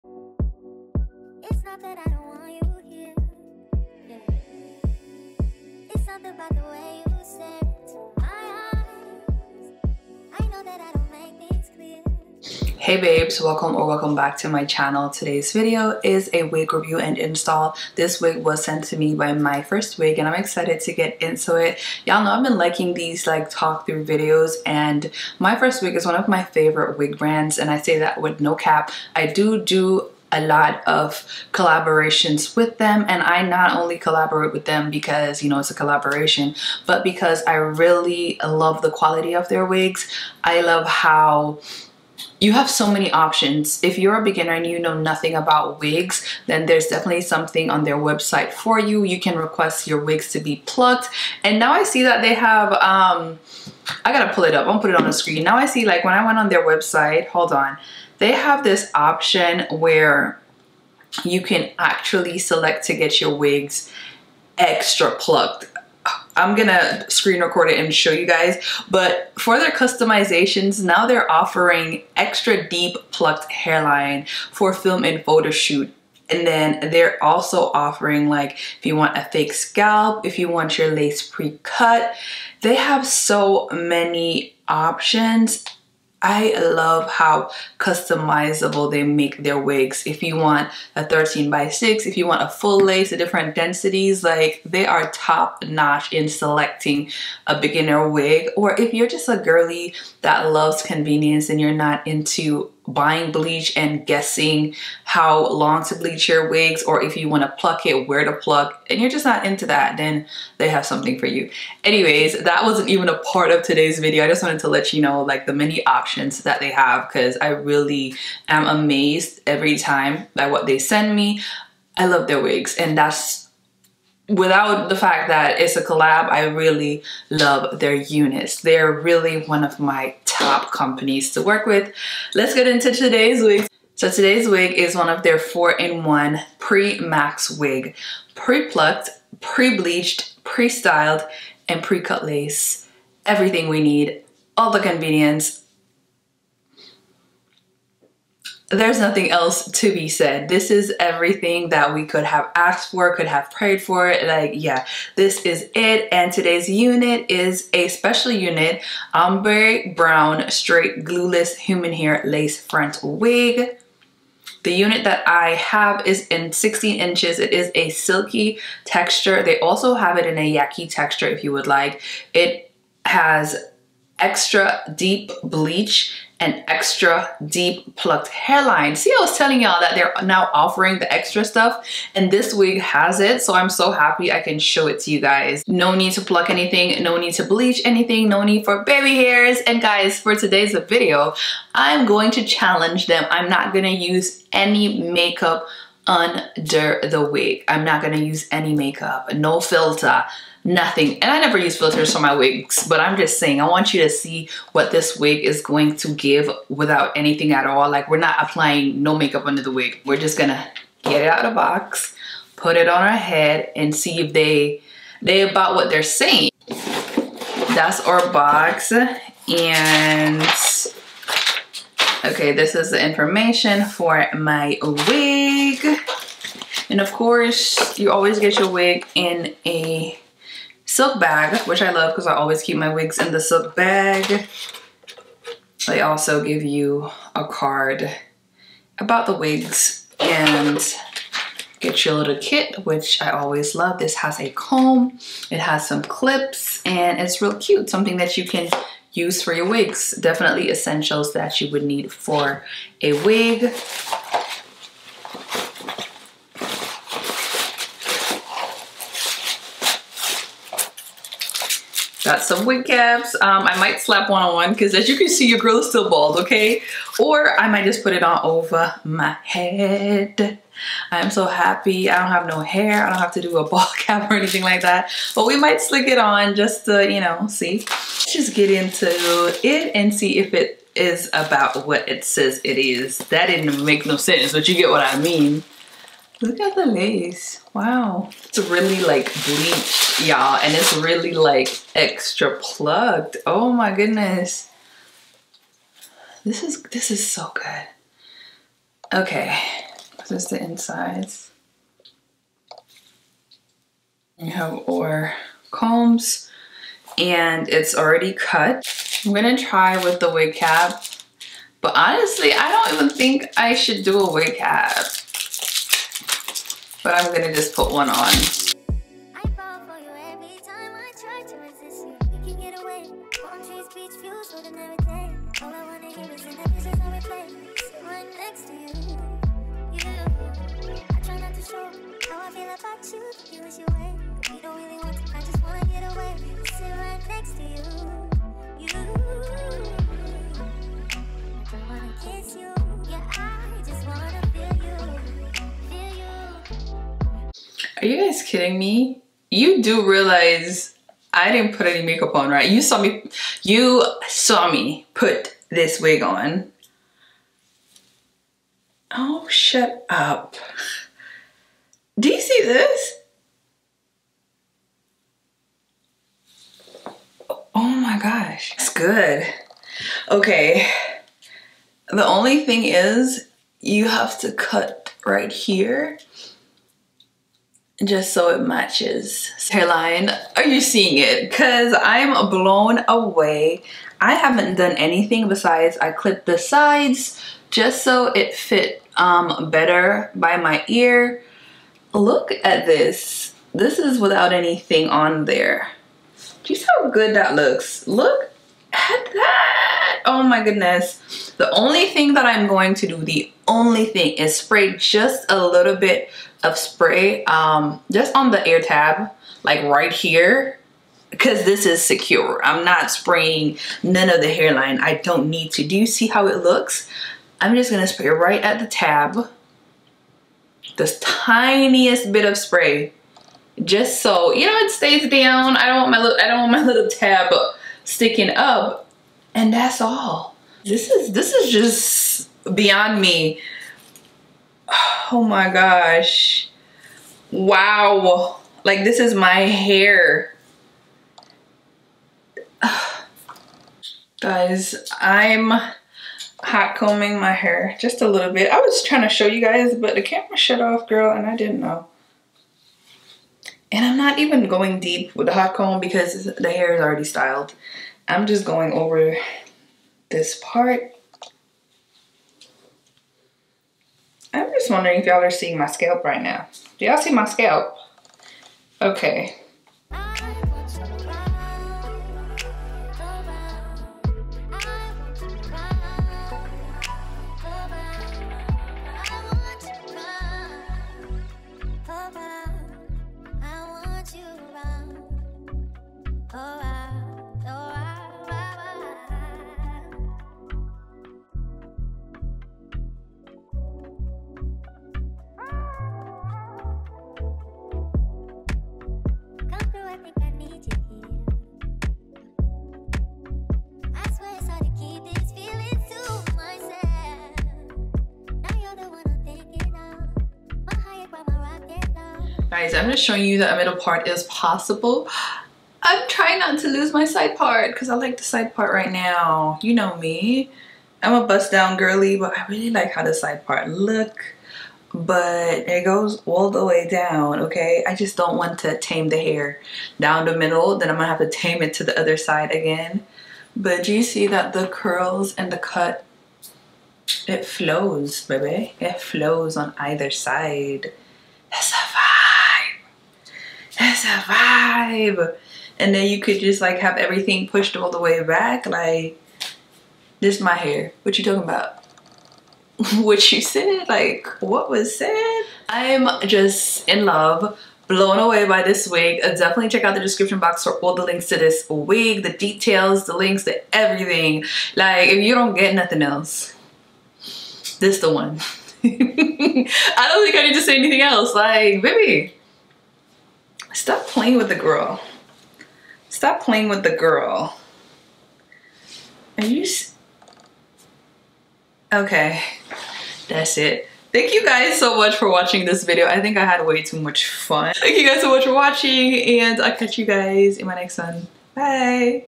NO GLUE, NO LACE LINE ! GIRL WHAT LACE??THE 4 IN 1 PRE- MAX WIG YOU NEED -FTMYFIRSTWIG
𝟰𝗶𝗻𝟭 𝗣𝗿𝗲-𝗠𝗮𝘅 𝗪𝗶𝗴!
√ Pre-plucked, Pre-bleached, Pre-cut Lace, Pre-styled
√ Invisible Lace
√ No shedding, No tangling, Long lifespan
√ No Glue & No Baby Hair Needed

Wig name: Aspecialunit- Ombre Brown Straight Glueless Human Hair Lace Front Wig - SPE085
Cap Construction: 13x6 Lace Front Wig
Hair Texture: Silky
Hair length: 16 Inch
Pre-cut Lace, Extra Deep Plucked Hairline & Extra Deep Bleached